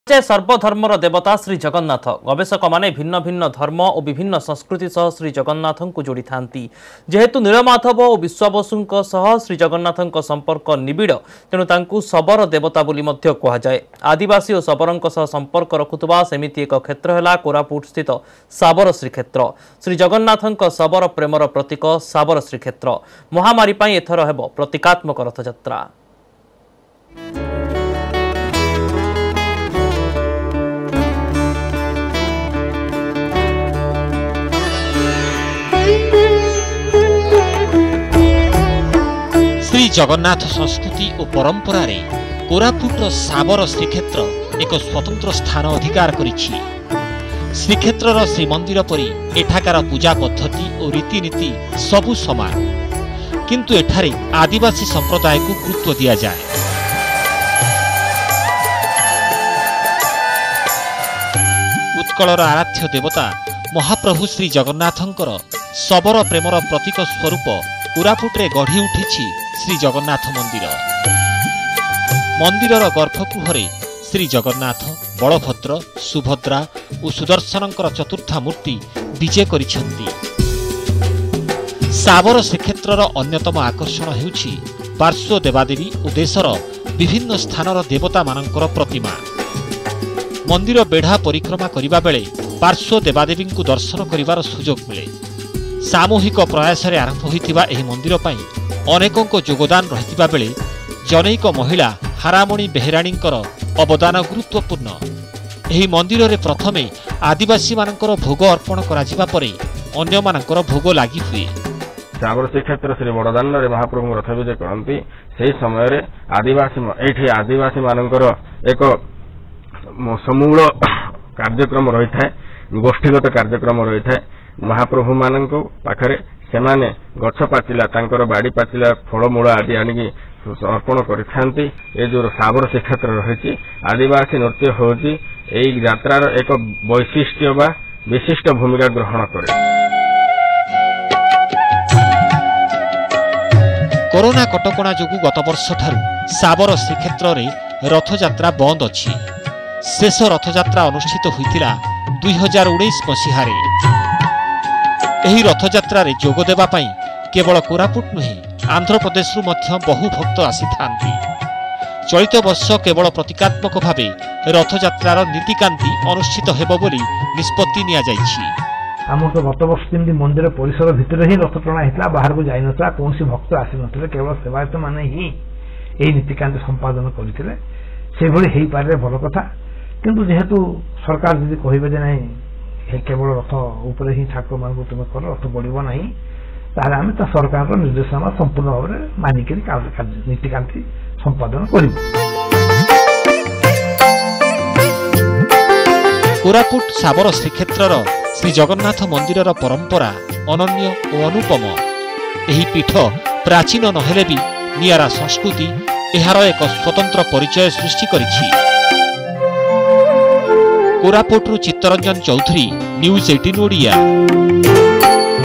Koraput: ଶାବର ଶୀକ୍ଷେତ୍ରରେ ଏଥର ହେବ ପ୍ରତିକାତ୍ମକ ରଥଯାତ୍ରା
ଜଗନ୍ନାଥଙ୍କ ଶବର ପ୍ରେମର ପ୍ରତୀକ ଶବର ଶ୍ରୀକ୍ଷେତ୍ର | ଚଳିତ ବର୍ଷ ମହାମାରୀ ପାଇଁ Koraput ଶାବର ଶୀକ୍ଷେତ୍ରରେ ଏଥର ହେବ ପ୍ରତିକାତ୍ମକ ରଥଯାତ୍ରା | 5th July 2021